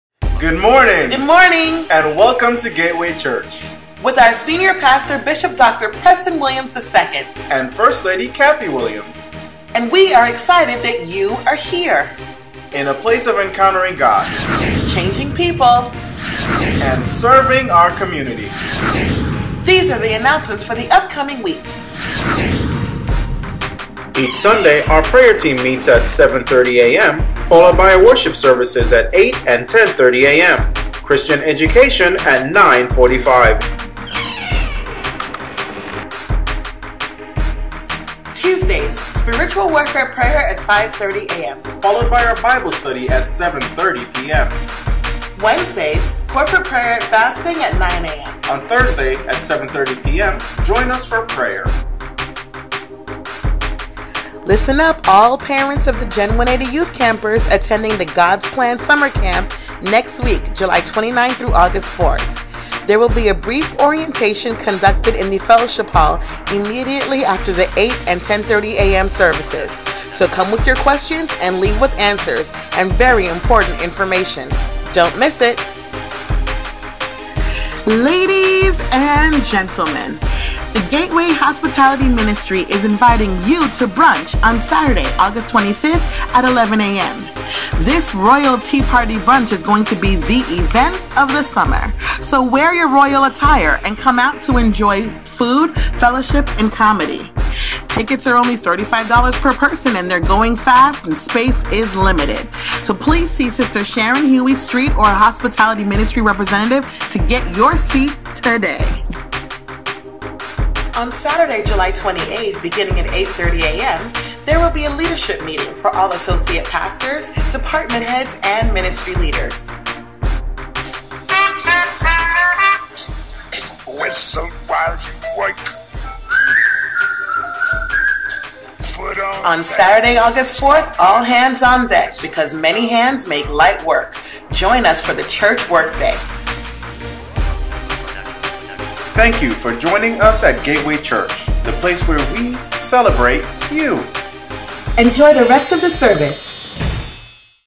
July 22 2018 VA
This video is about July 22nd weekly video announcement for Gateway Church.  Bishop Dr. Preston Williams II